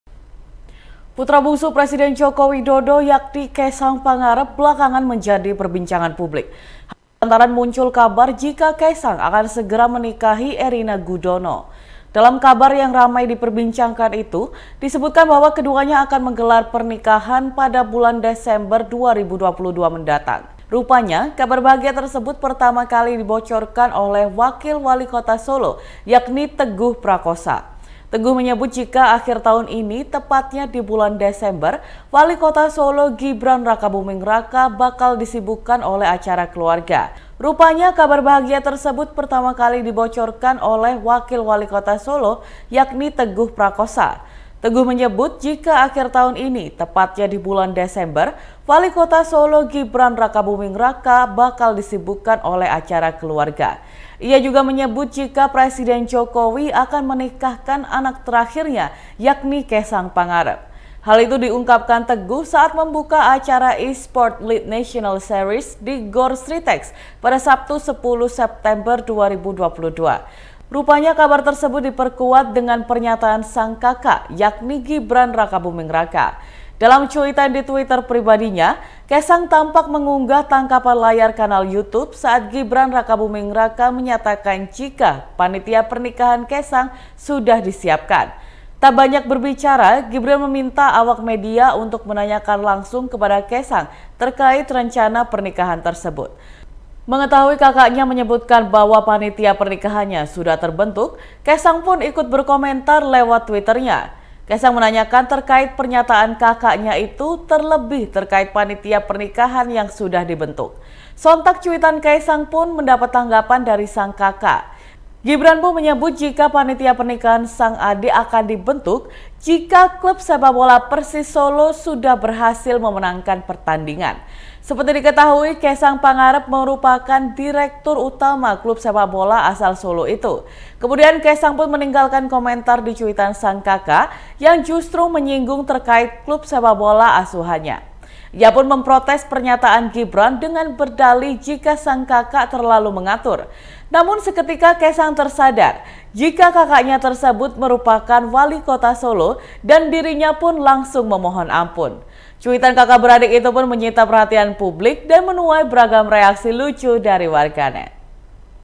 RESPONS KAESANG PANGAREP Saat Rencana Pernikahan Dibongkar di Acara E-Sport
Putra bungsu Presiden Joko Widodo, yakni Kaesang Pangarep belakangan menjadi perbincangan publik.

Hal itu lantaran muncul kabar jika Kaesang akan segera menikahi Erina Gudono.

Dalam kabar yang ramai diperbincangkan itu, disebutkan bahwa keduanya akan menggelar pernikahan pada bulan Desember 2022 mendatang.

Rupanya, kabar bahagia tersebut pertama kali dibocorkan oleh Wakil Wali Kota Solo, yakni .

Teguh menyebut jika akhir tahun ini tepatnya di bulan Desember, Walikota Solo Gibran Rakabuming Raka bakal disibukkan oleh acara keluarga.

Ia juga menyebut jika Presiden Jokowi akan menikahkan anak terakhirnya, yakni Kaesang Pangarep.

Hal itu diungkapkan Wakil Wali Kota Solo, Teguh Prakosa saat membuka acara E-Sport Lead National Series di GOR Sritex pada Sabtu (10/9/2022).